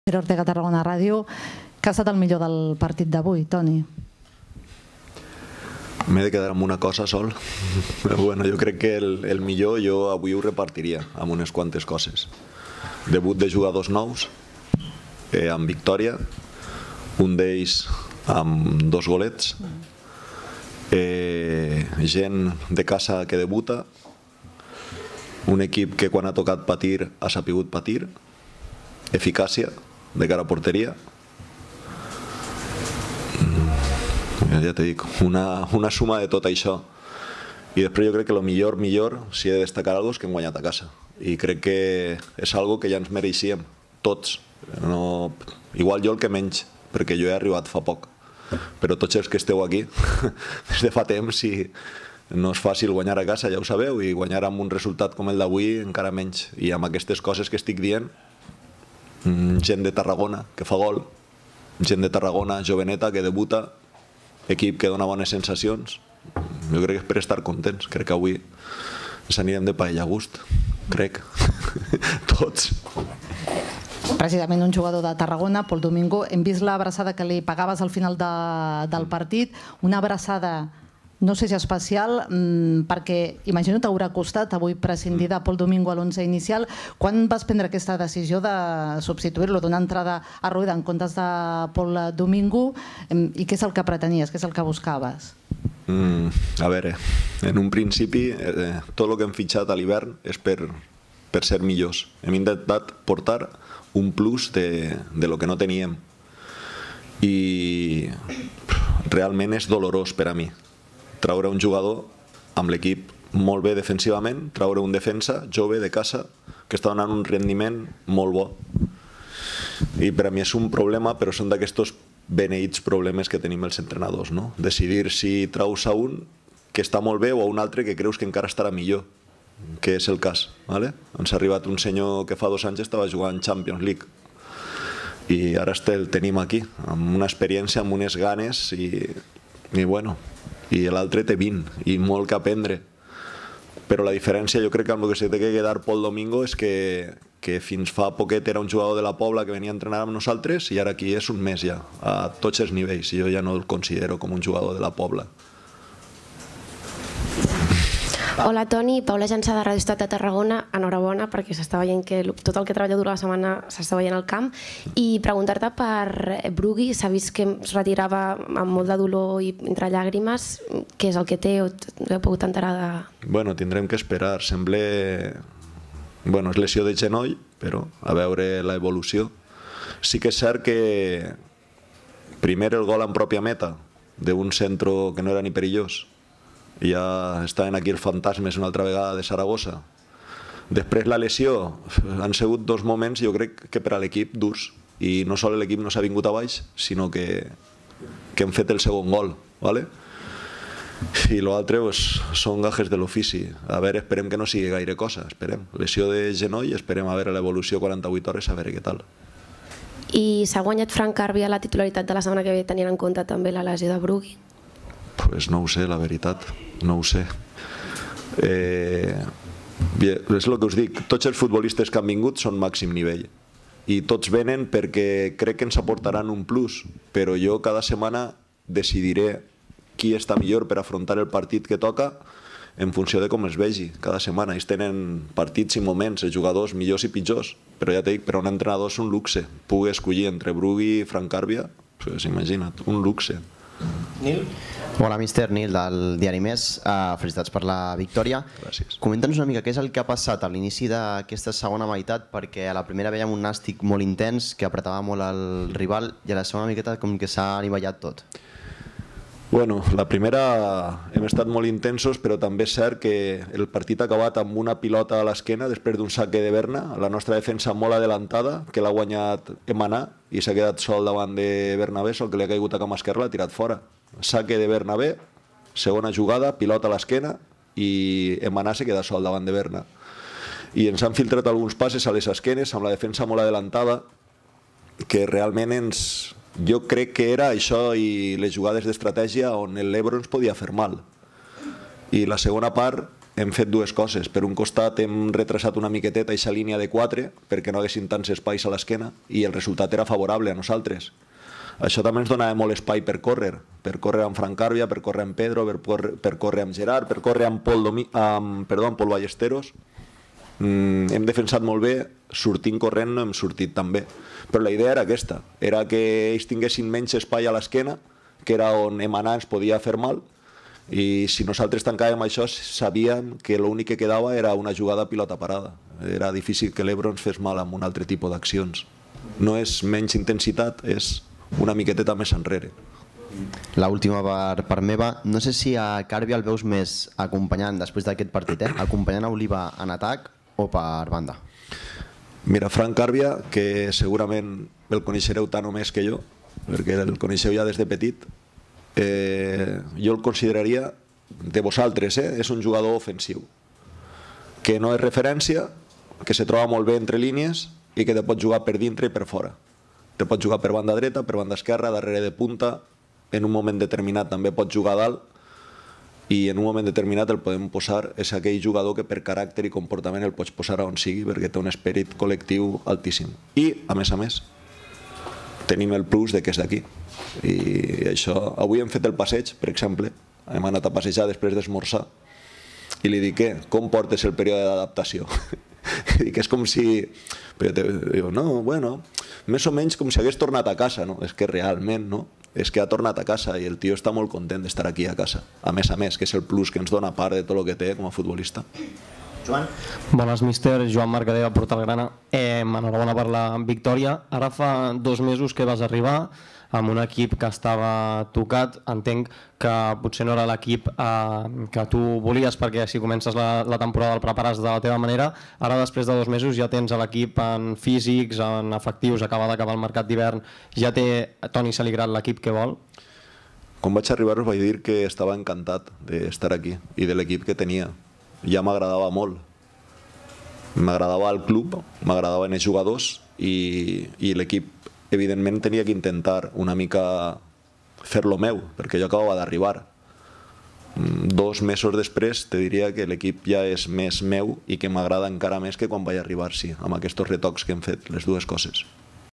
RDP de Toni Seligrat posterior a la victòria davant el CD Ebro per 4-1
El tècnic valencià ha valorat la importància de la victòria al Nou Estadi amb els doblets golejadors dels tarragonins Fran Carbia i Gerard Oliva